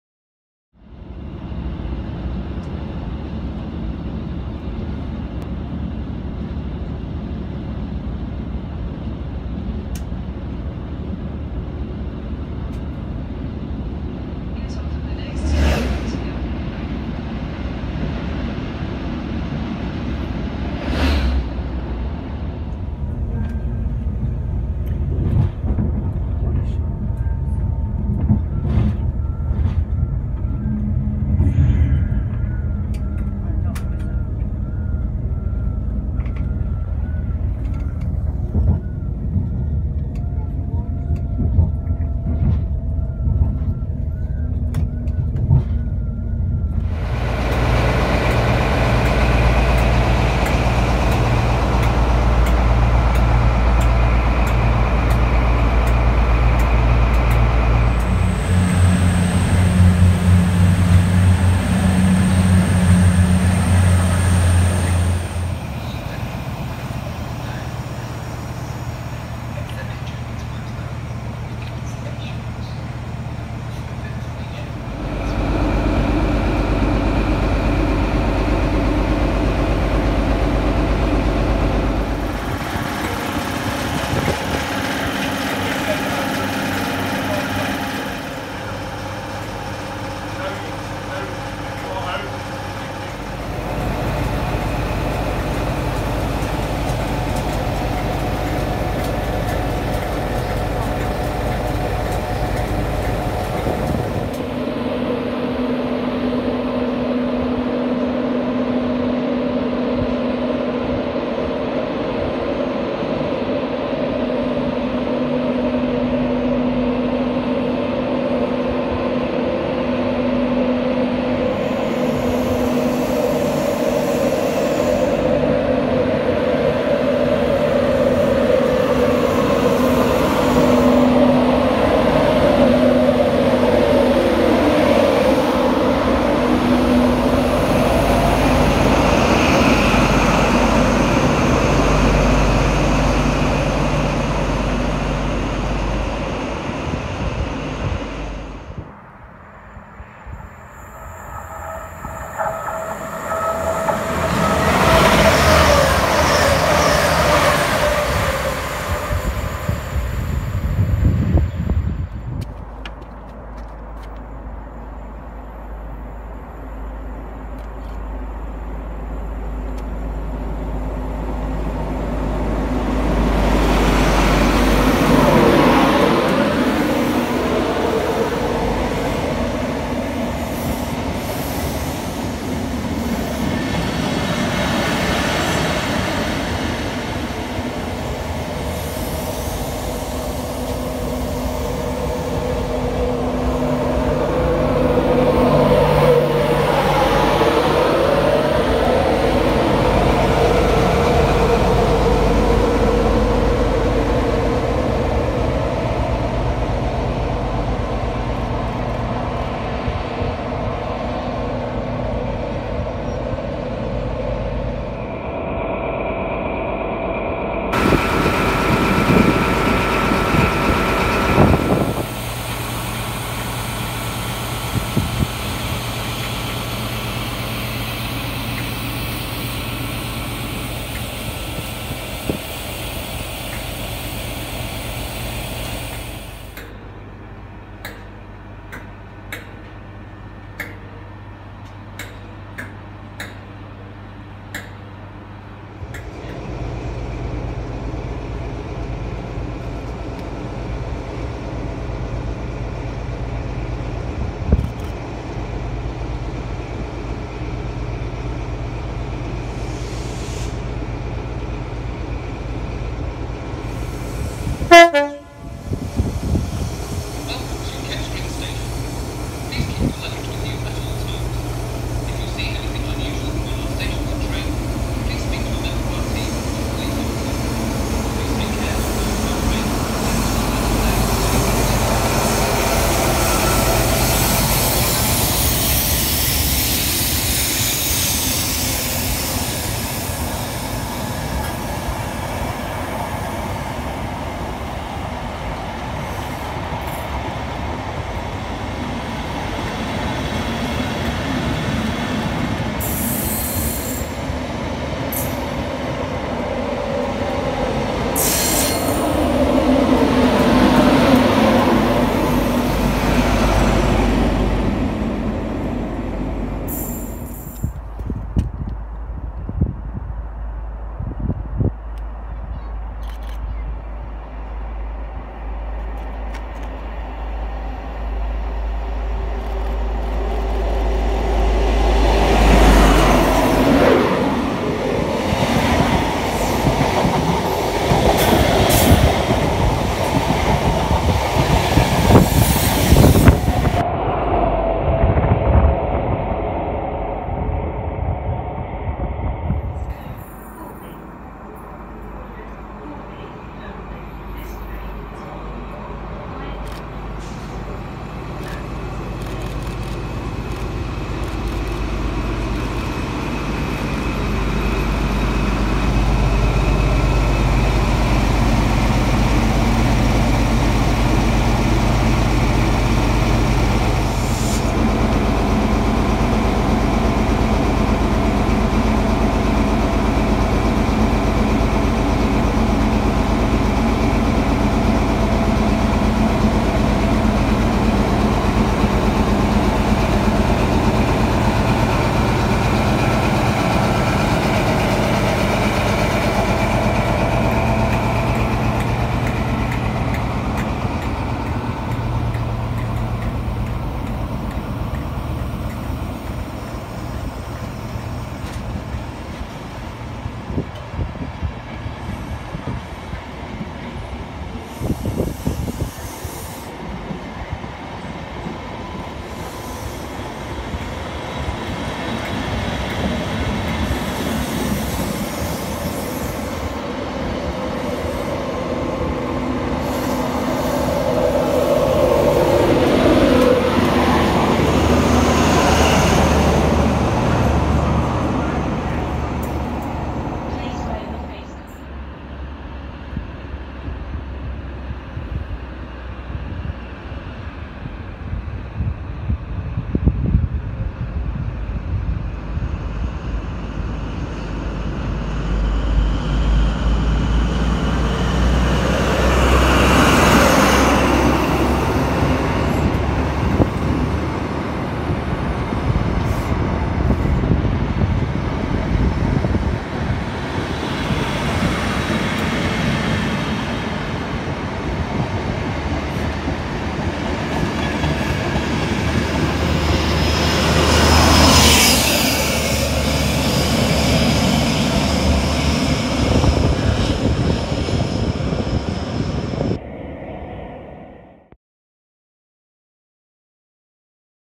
DATS Test Train Action at Kettering
In this video Im at Kettering on the Midland Mainline.

I solely went for the DATS/ROG test train that's running between Kettering and Bedford and Corby and Elstow utilising Ex EMR Power cars 43066 and 43054 and as an added bonus 91122, 91128 and 90035 which have never ran over these rails before under their own power.

it was great seen both diesel and electric locos working together but towards the end of the day 91128 suffered water ingress to its pantograph so was isolated and remained dead for the rest of its workings.

I stayed at Kettering the whole day but caught its movements head to and from Rectory Total Sidings which was nice to see.